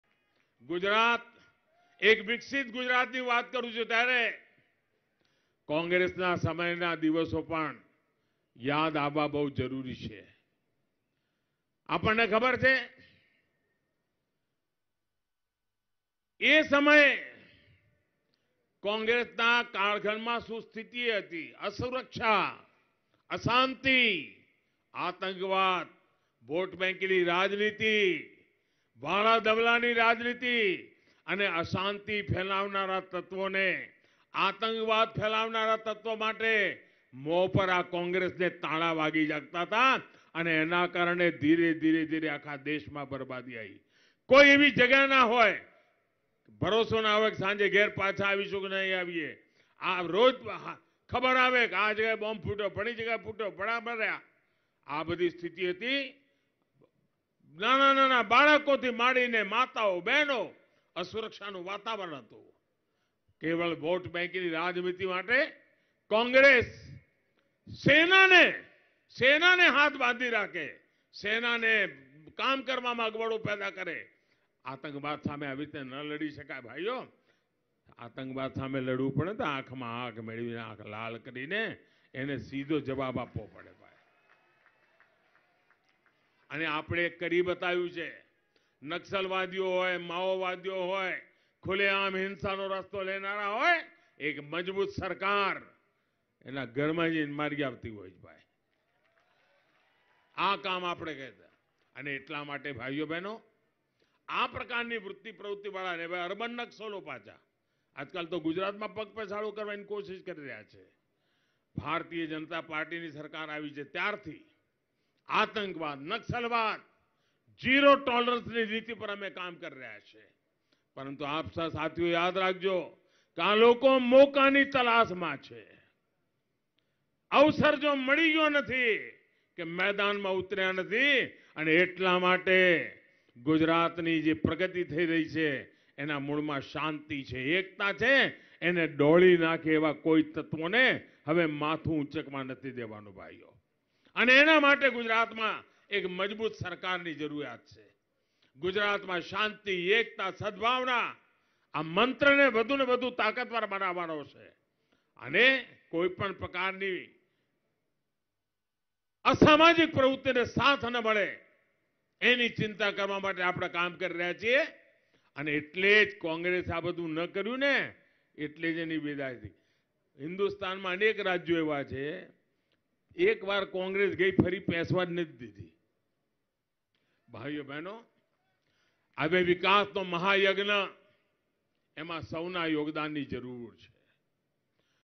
आतंकवाद और नक्सलवाद के समर्थकों को Special Treatment देने के दिन लद गए!
Addressing his third public meeting of the day, The Prime Minister said, “It is equally important for a developed India to be a self-reliant India. And that's why Gujarat's industries, MSMEs-small scale industries have a huge role to play. Jamnagar's brass industry and bandhani art have received a lot of support over the years. Today, Jamnagar produces everything from pins to aeroplane parts”.